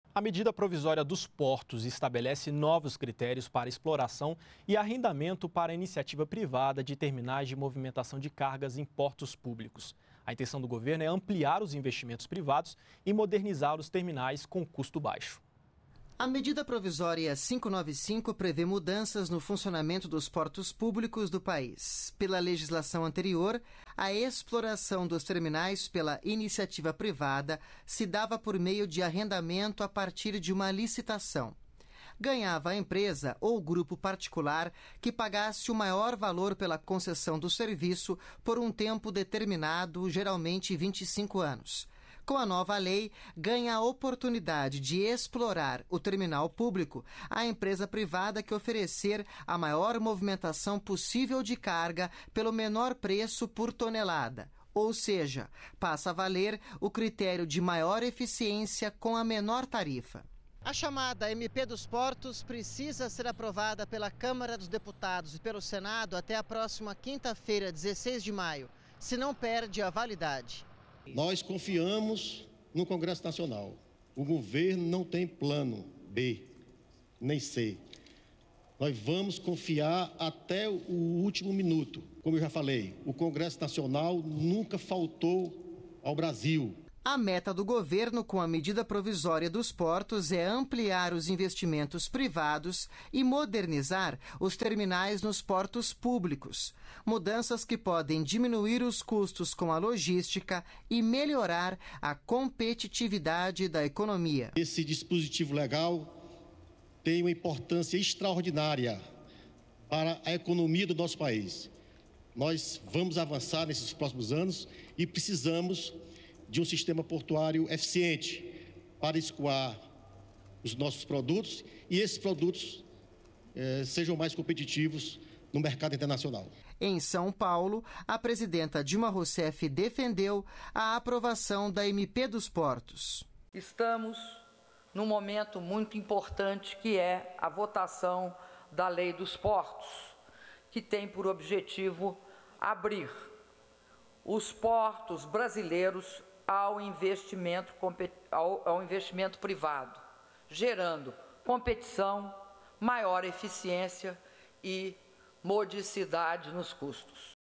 Objetivo da MP dos Portos é modernizar os terminais públicos
NBR NOTÍCIAS - 13.05.13: A Medida Provisória 595, chamada MP dos Portos, estabelece novos critérios para exploração e arrendamento de terminais de movimentação de cargas em portos públicos. A intenção do governo é ampliar os investimentos privados e modernizar os terminais com custo baixo. A legislação anterior, a exploração dos terminais pela iniciativa privada se dava por arrendamento a partir de licitação. Ganhava a empresa ou grupo particular que pagasse o maior valor pela concessão do serviço por um tempo determinado, geralmente de 25 anos. Com a nova lei, a empresa privada que oferecer a maior movimentação de carga pelo menor preço por tonelada ganha a oportunidade de explorar o terminal público. Dessa forma, passa a valer o critério de maior eficiência com a menor tarifa. Para não perder a validade, a MP dos Portos precisa ser aprovada pela Câmara dos Deputados e pelo Senado até a próxima quinta-feira, 16 de maio.